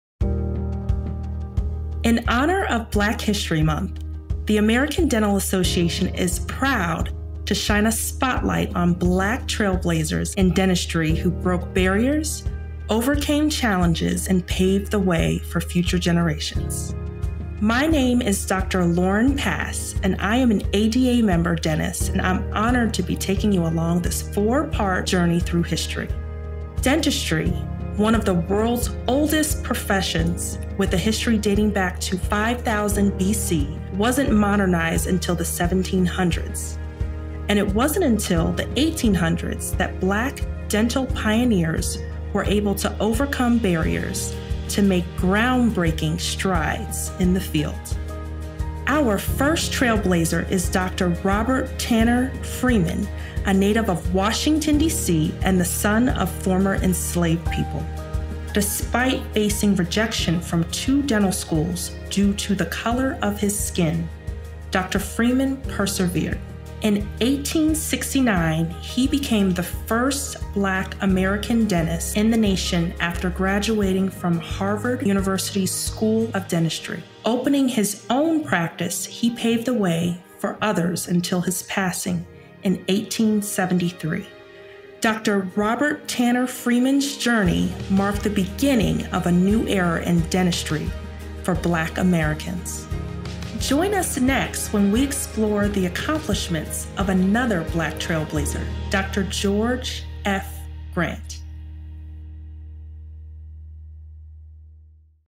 Black Trailblazers in Dentistry: Dr. Robert Tanner Freeman
We’re celebrating BlackHistoryMonth by honoring the incredible Black dentists who have paved the way in the field of dentistry. From breaking barriers to making significant contributions, their legacy continues to inspire us all.

*photo courtesy of Harvard University